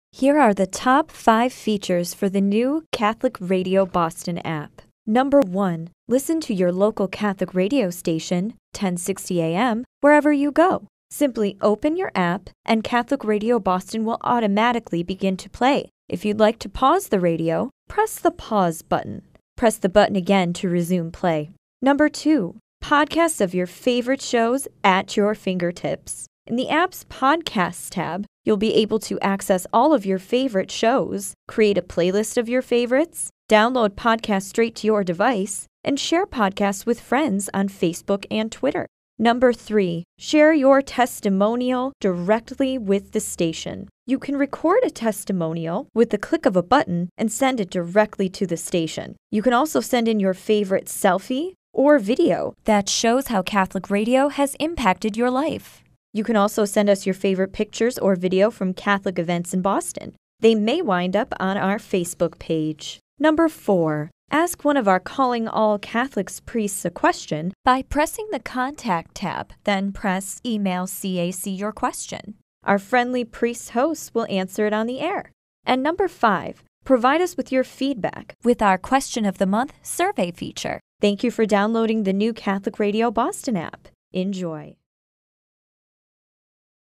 The Top 5 Features of the NEW Catholic Radio Boston App
The Catholic Radio Boston App is a brand new dynamic digital experience that allows Boston residents to experience Catholic Radio like never before.